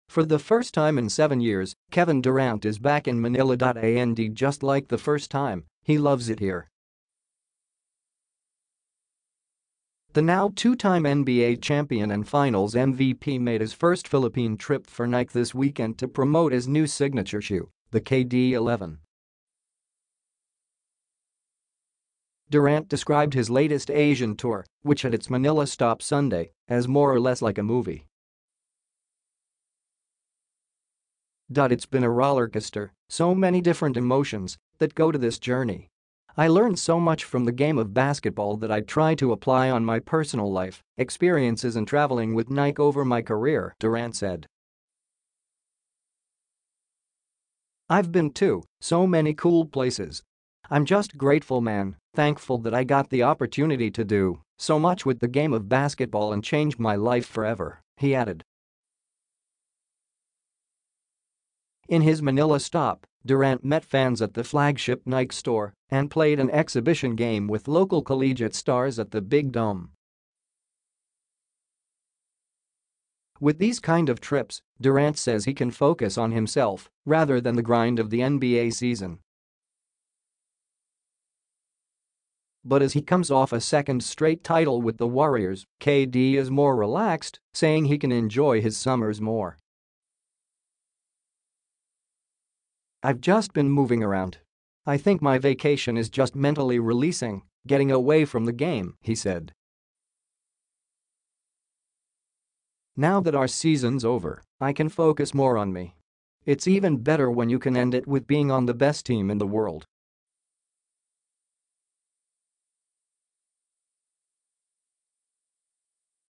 KDinMNL: More relaxed Durant puts on memorable show for Pinoy fans
For the first time in seven years, Kevin Durant is back in Manila.

And just like the first time, he loves it here.

The now two-time NBA champion and Finals MVP made his first Philippine trip for Nike this weekend to promote his new signature shoe, the KD 11.

Durant described his latest Asian tour, which had its Manila stop Sunday, as more or less like a "movie."

"It’s been a rollercoaster, so many different emotions that go to this journey. I learned so much from the game of basketball that I try to apply on my personal life, experiences and traveling with Nike over my career," Durant said.

"I’ve been to so many cool places. I’m just grateful man, thankful that I got the opportunity to do so much with the game of basketball and change my life forever," he added.

In his Manila stop, Durant met fans at the flagship Nike store and played an exhibition game with local collegiate stars at the Big Dome.

With these kind of trips, Durant says he can focus on himself rather than the grind of the NBA season.

But as he comes off a second straight title with the Warriors, KD is more relaxed, saying he can enjoy his summers more.

"I’ve just been moving around. I think my vacation is just mentally releasing, getting away from the game," he said.

"Now that our season’s over, I can focus more on me. It’s even better when you can end it with being [on] the best team in the world. It’s pretty cool going into the summer being that," Durant added.